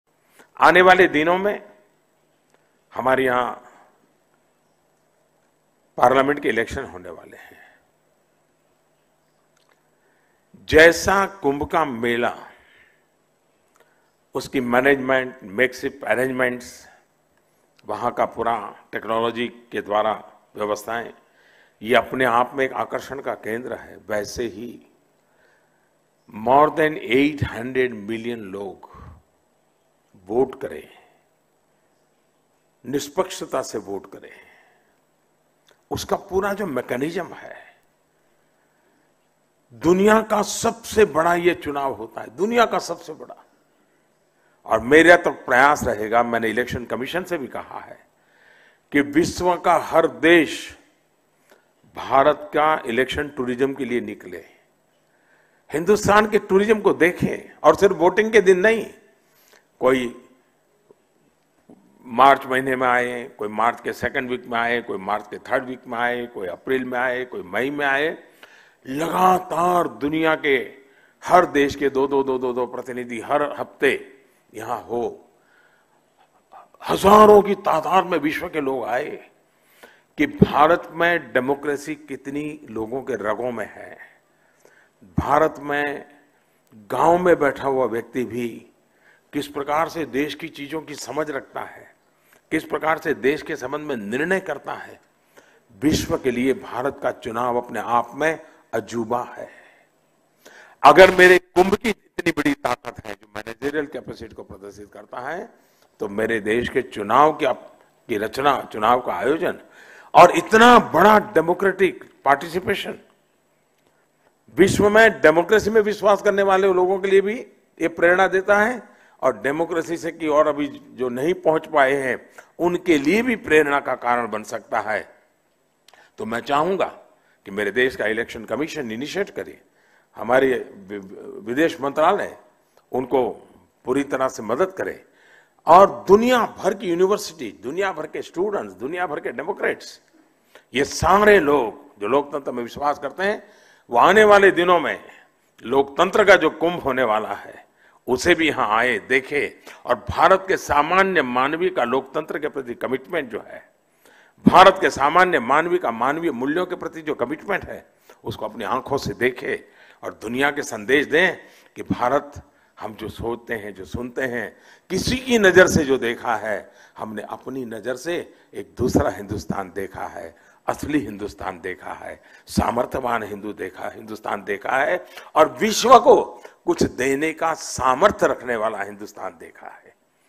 India's parliamentary elections are the "Kumbh of democracy": PM
Prime Minister Narendra Modi with Pravasi Bharatiya representatives from 190 countries